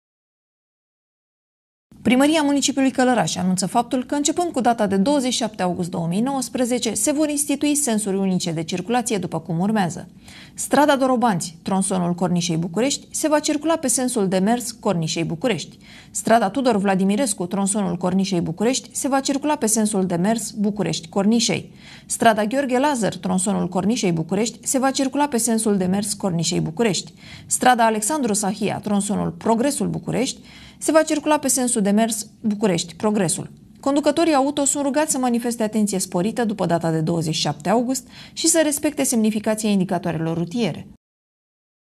Știri Locale - Străzi cu sens unic din 27 august
Știri Locale

Primăria Municipiului Călărași anunță faptul că, începând cu 27 august 2019, se vor institui sensuri unice de circulație.
Conducătorii auto sunt rugați să manifeste atenție sporită după data de 27 august și să respecte semnificația indicatoarelor rutiere.